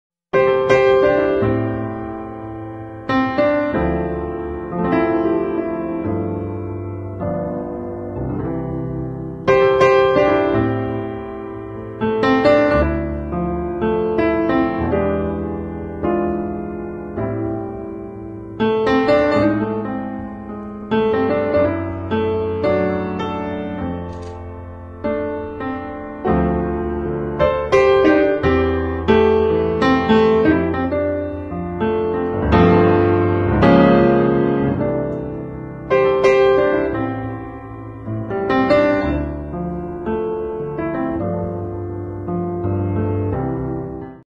Haute Couture svadobné šaty od Galia Lahav
Miss Slovensko 2017 v Haute Couture svadobných šatách od Galia Lahav. Fotenie pre Galia Lahav Trunk Show.